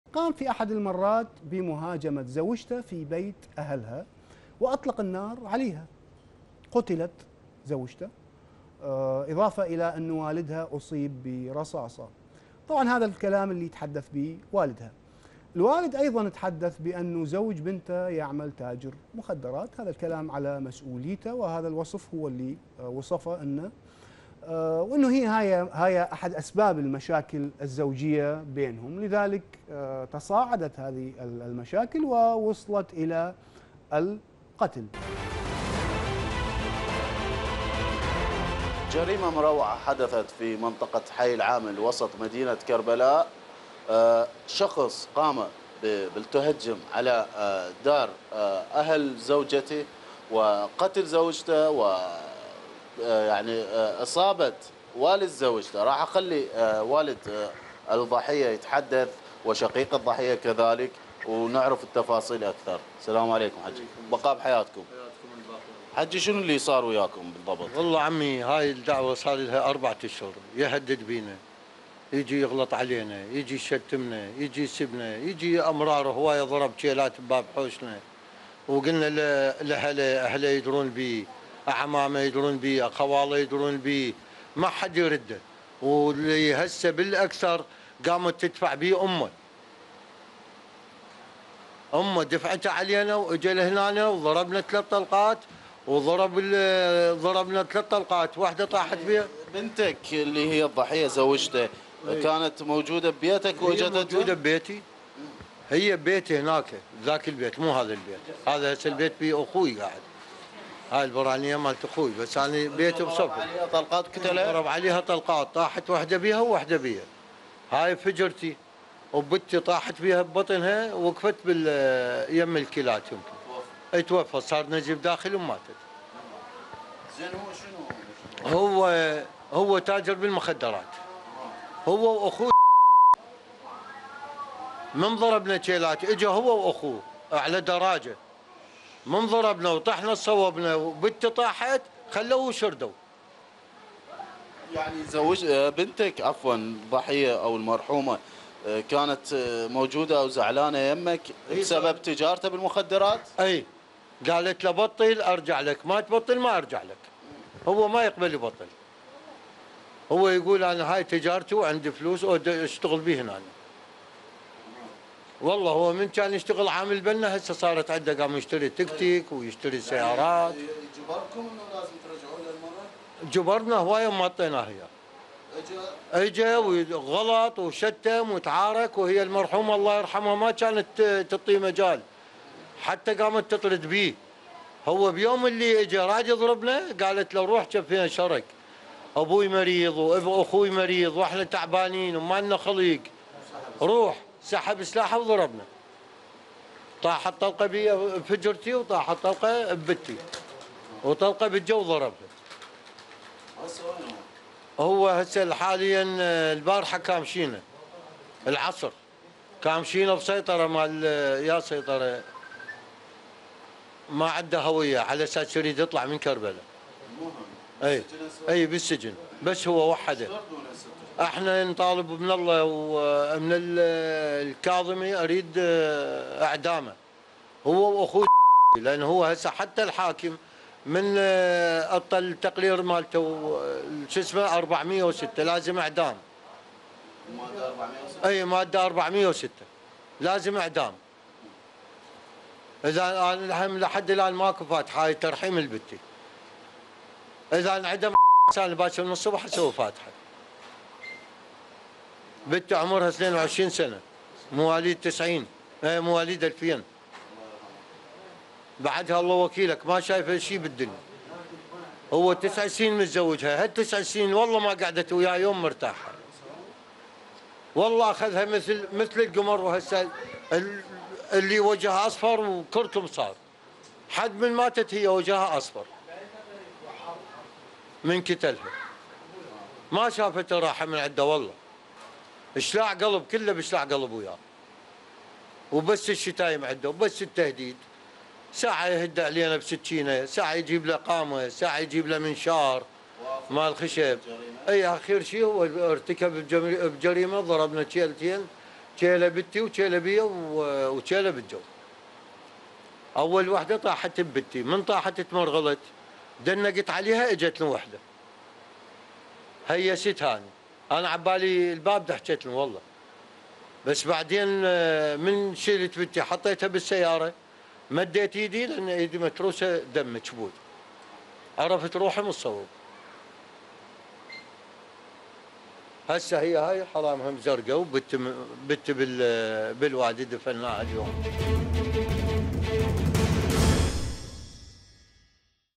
أغرب مشكلة زوجية في العراق بحاجة إلى تدخل جهاز المكافحة
موقع قناة التغيير الفضائية الالكتروني